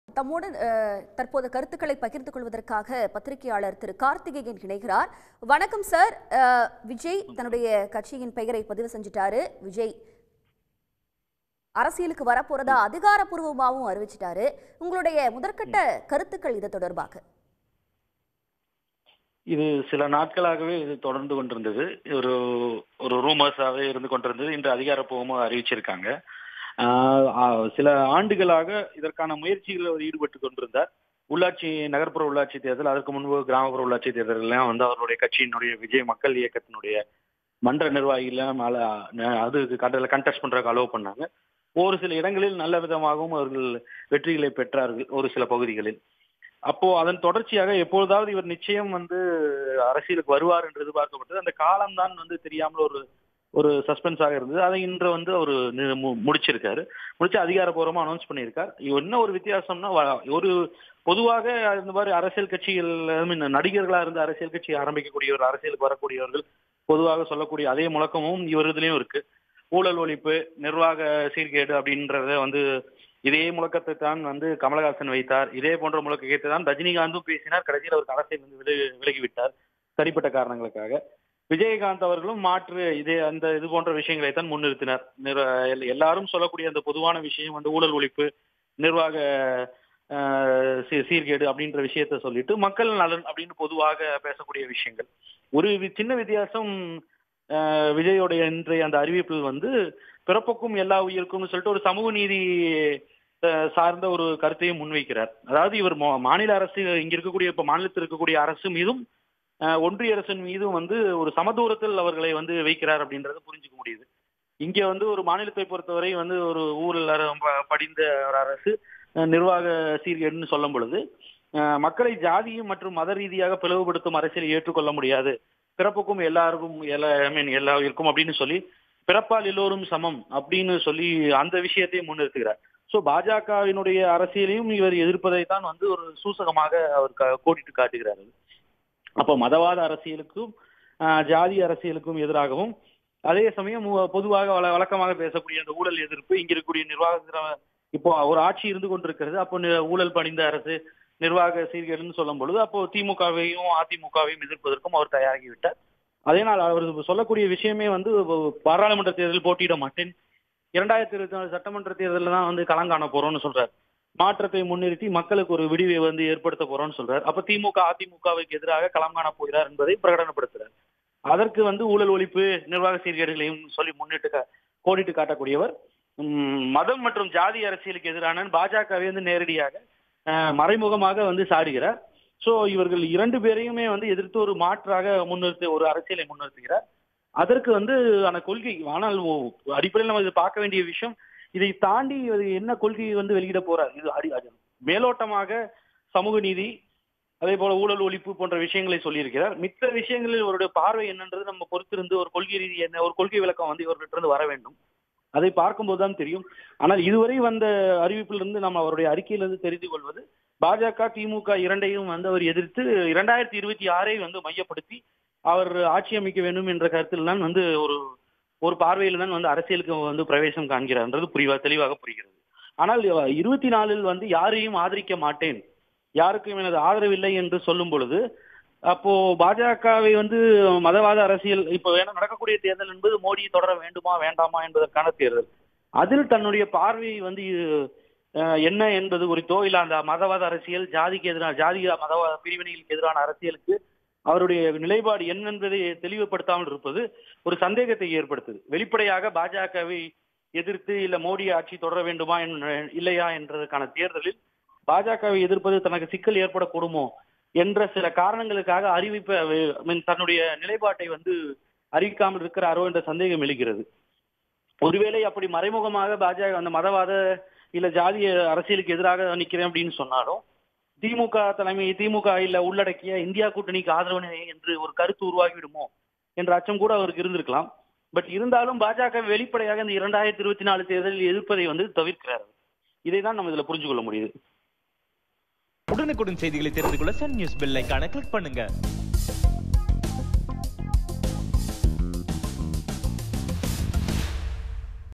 BREAKING | BJPயை எதிர்க்கிறாரா Vijay? தவெக கட்சி பெயரில் இருக்கும் ரகசியம்! - Karthikeyan | Sun News
BREAKING | BJPயை எதிர்கிறாரா Vijay? தவெக கட்சி பெயரில் இருக்கும் ரகசியம்! - Karthikeyan | Sun News